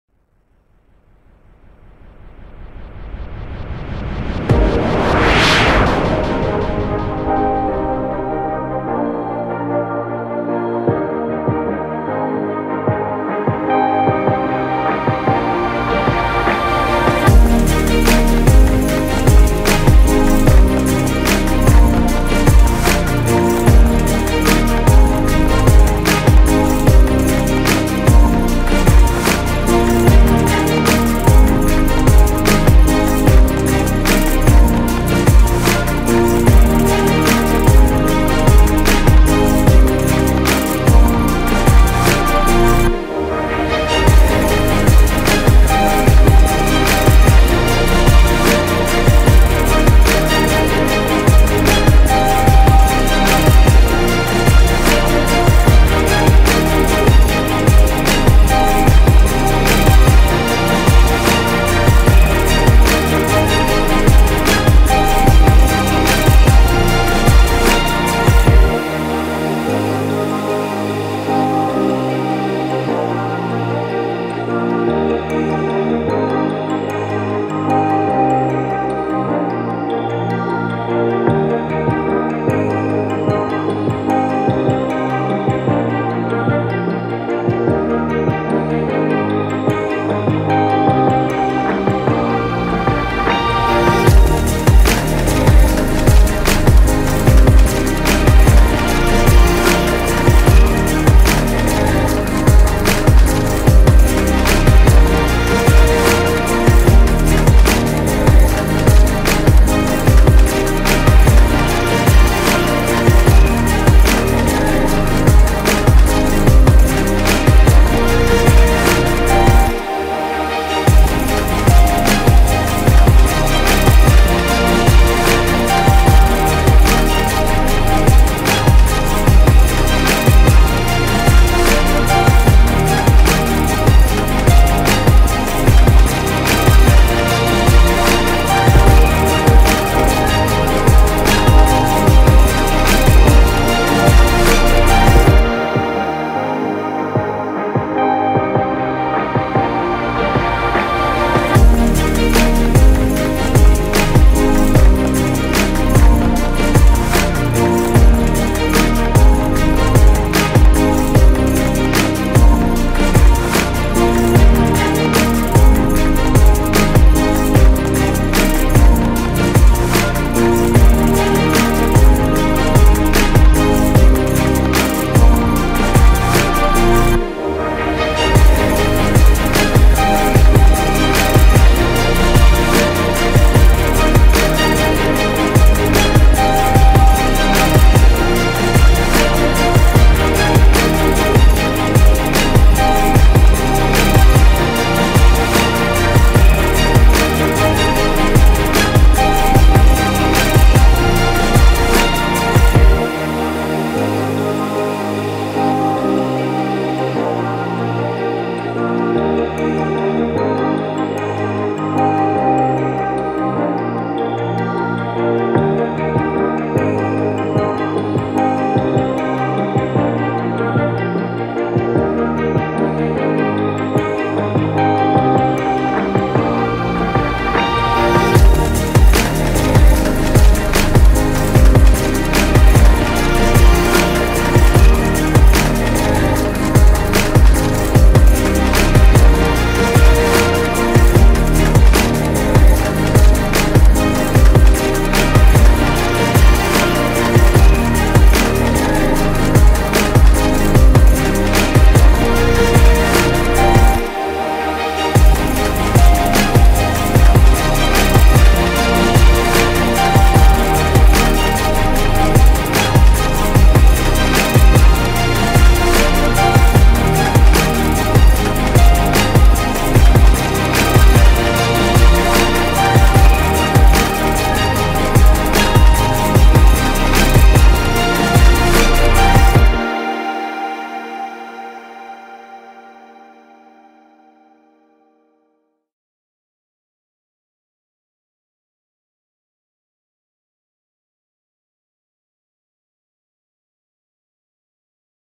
RAJON RONDO DRAWING I colored pencil drawing
Here is a simple colored pencil drawing of one of my favorite point guard in the NBA Rajon Rondo. I just used ordinary colored pencils on this artwork hopefully i could use prisma color next time.